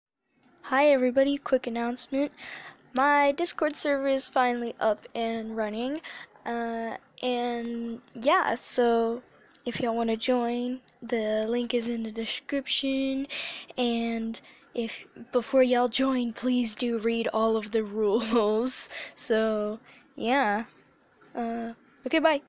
My new discord server is finally here
Hey everyone! My new and improved discord server is up!
Before you join, please note that you must be 16 years or older to join my server. I will only make acepions for close friends and family.
Once you join please read all of the rules that I have made.
Now have fun!